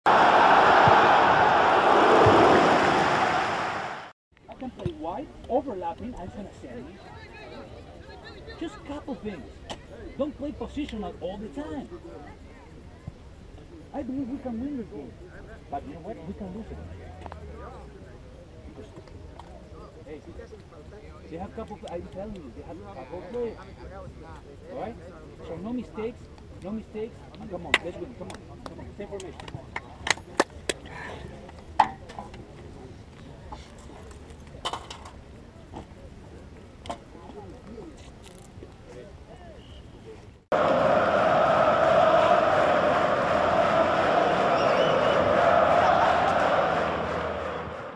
TOCATV: HS Boys SPIDERS (Half Time - Coach's Words)
MSI Classic
Sunday, October 2, 10:40 AM
FC Atletico Seneca vs HS Boys SPIDERS
Soccerplex #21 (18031 Central Park Circle, Boyds, MD 20841)